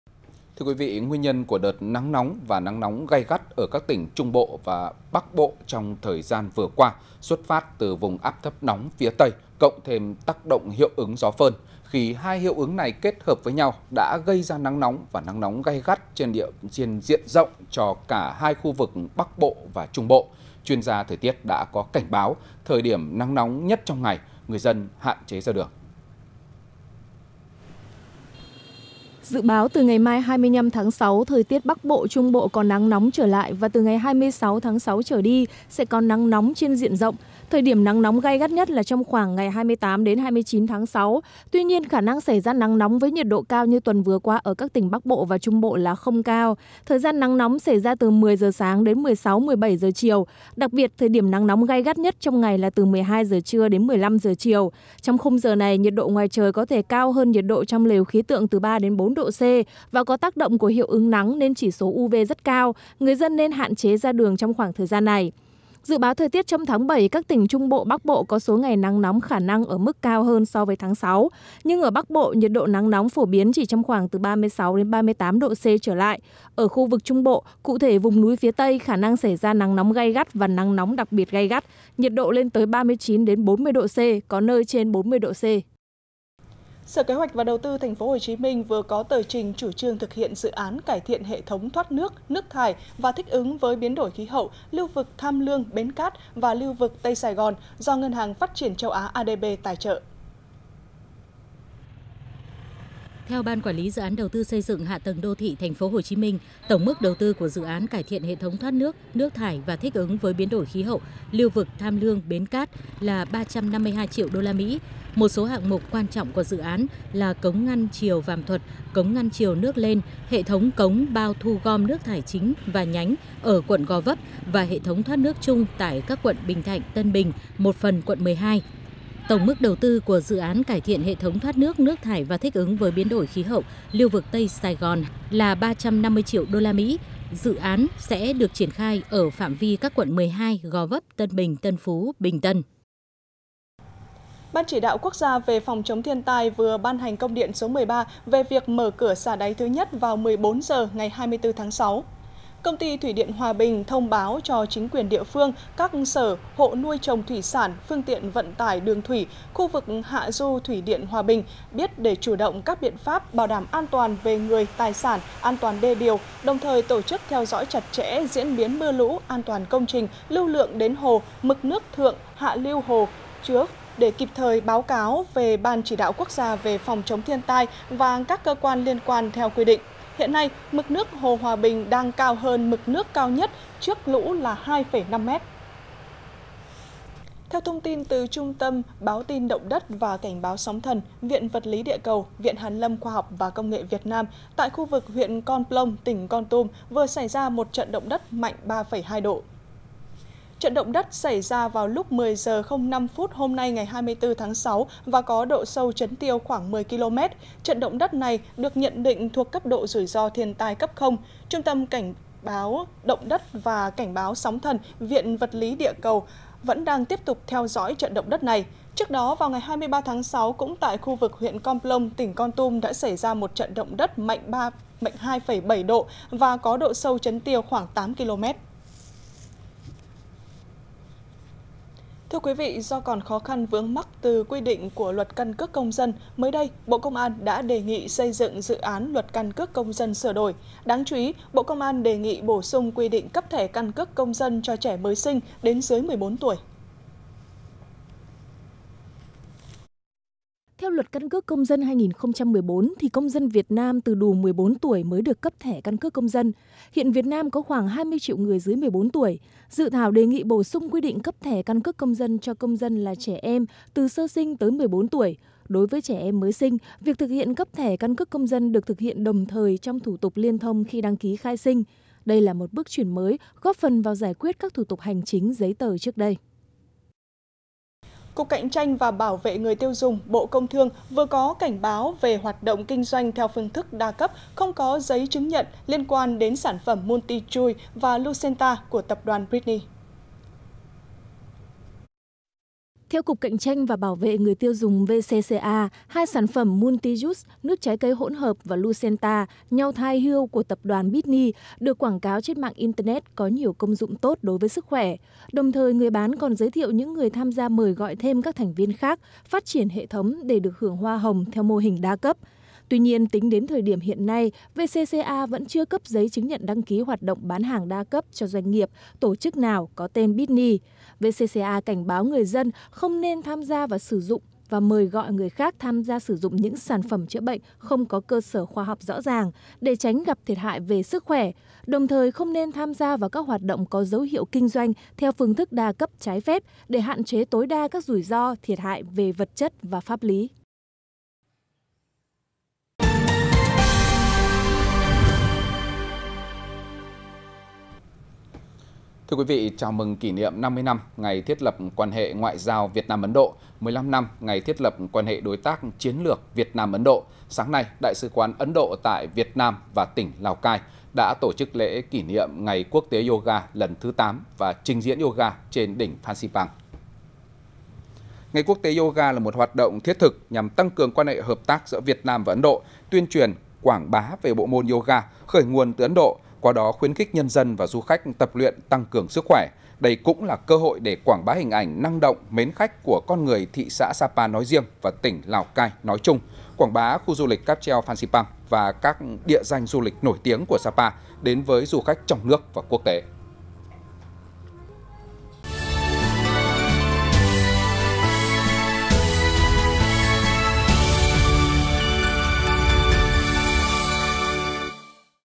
Cuối tháng 6, đầu tháng 7 tiếp tục nắng nóng gay gắt | Tin tức đời sống, xã hội tối ngày 24/6
Cuối tháng 6, đầu tháng 7 tiếp tục nắng nóng gay gắt | Tin tức đời sống, xã hội tối ngày 24/6 | Truyền Hình Nhân Dân
1. Dự Báo Thời Tiết Hôm Nay